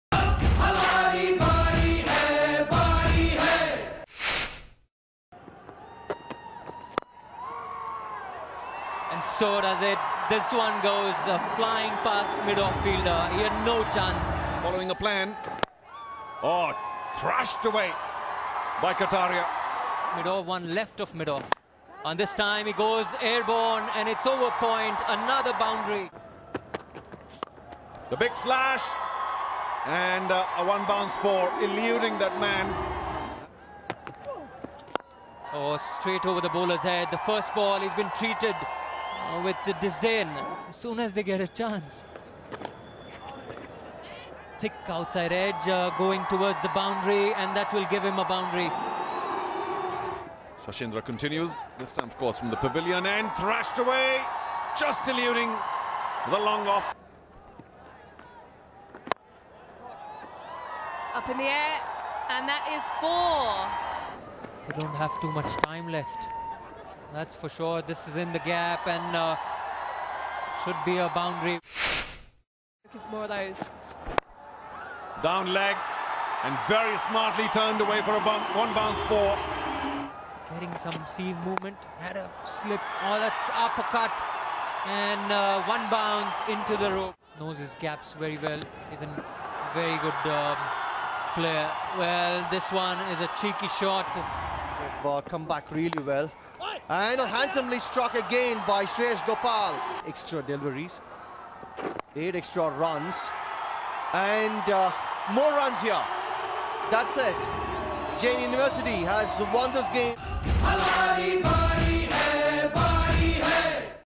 Jain University vs Aligarh University: Sizzling Fours
In a low scoring encounter at the Chinnaswamy Stadium in Bengaluru, Yasir Kaleem top scored for Aligarh with 30, including four boundaries. For the hosts, skipper Kaunain Abbas was the star yet again, notching four boundaries in an unbeaten 44, ultimately taking his side to victory.